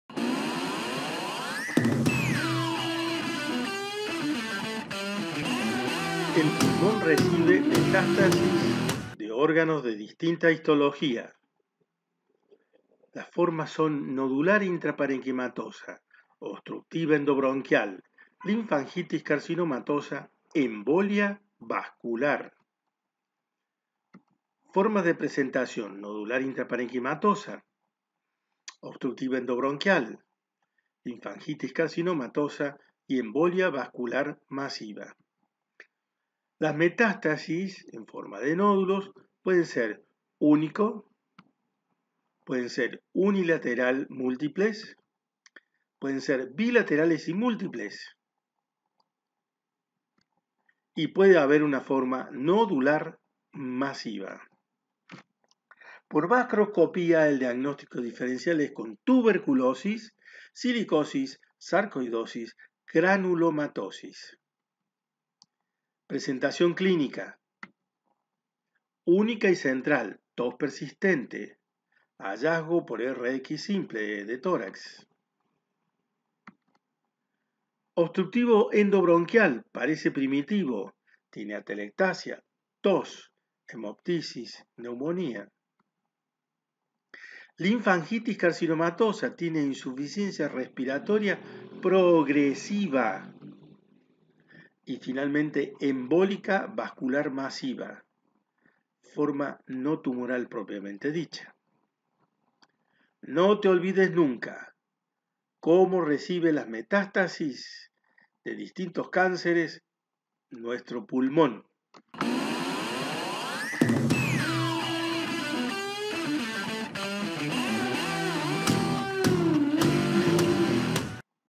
SINTESIS PULMON RECIBE METASTASIS
ORGANO CLAVE EN LA EVOLUCION DE LOS DISTINTOS CANCERES - SINTESIS PULMON RECIBE METASTASIS DE DISTINTO ORIGEN HISTOLOGICO - MODALIDAD DE PRESENTACION Y DIAGNOSTICOS DIFERENCIALES